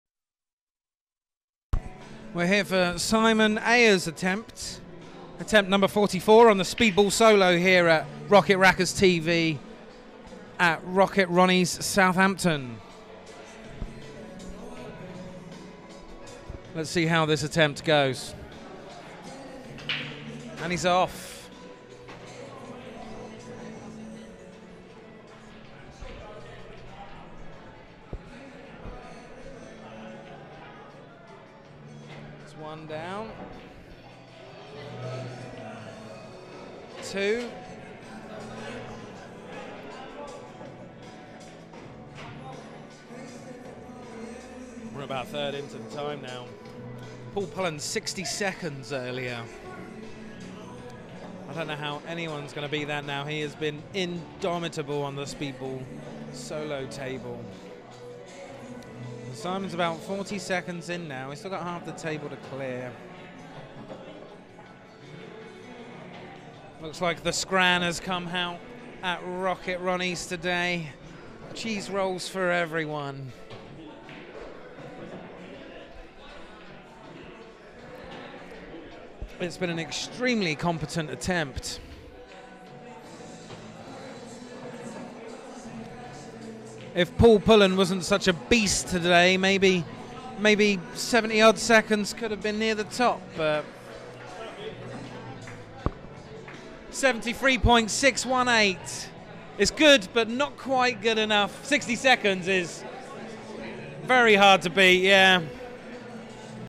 I don't understand Speed Pool | Matt Scannell Charity Fundraiser 2021
Clearly I 'didn't get the assignment' as the youngin's say.

For some reason I thought I had to wait until all the balls stopped, but they were doing the Ultimate Pool rules where you only have to wait for the cue ball and particular object ball to stop moving.

Oh well, at least I didn't miss!

The charity day was organised in memory of a lovely guy by the name of Matt Scannell who is deeply missed by the Pool community in Southampton.

You can see the entire day's Speed Pool challenge on the wonderful Rocket Racker's TV channel: